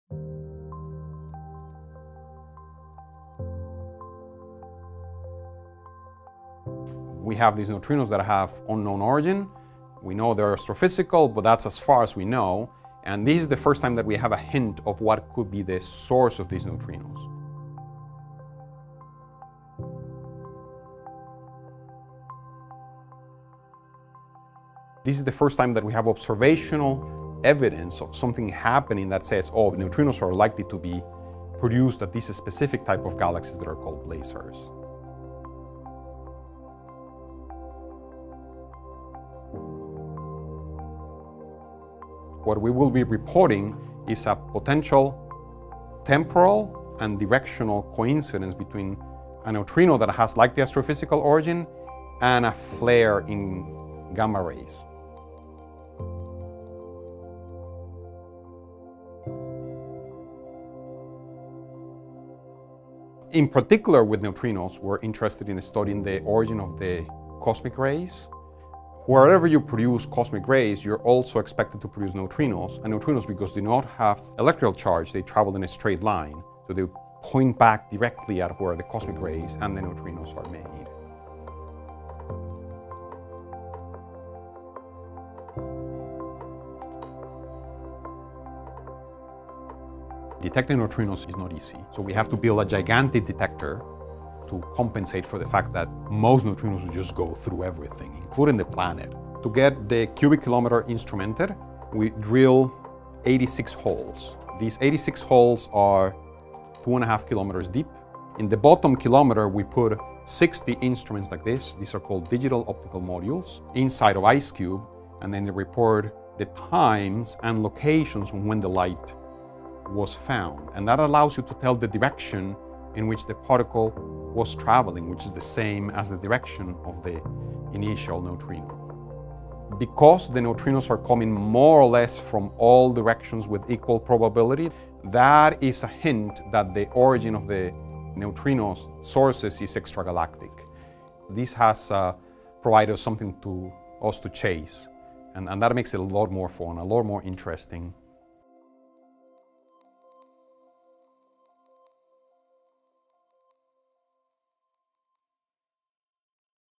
Ice Cube and the Ghost Particle
IceCube Neutrinos Point to Long-Sought Cosmic Ray Accelerator

An international team of scientists, including two researchers from Georgia Tech, has found the first evidence of a source of high-energy cosmic neutrinos, ghostly subatomic particles that can travel unhindered for billions of light years from the most extreme environments in the universe to Earth. The observations, made by the IceCube Neutrino Observatory at the Amundsen–Scott South Pole Station and in coordination with telescopes around the globe and in Earth’s orbit, help resolve a more than a century-old riddle about what sends subatomic particles such as neutrinos and cosmic rays speeding through the universe.